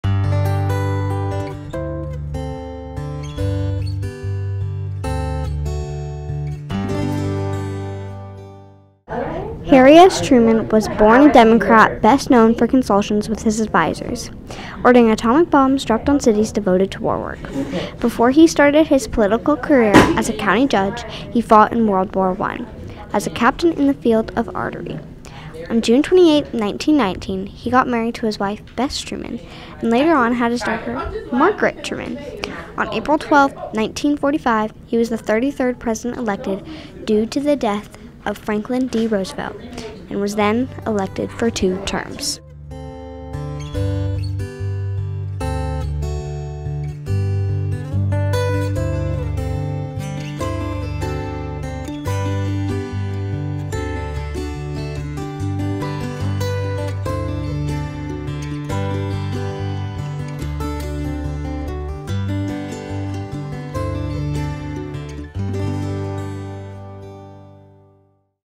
Harry Truman
This is an entry in the Project POTUS Film Festival about President Harry S Truman.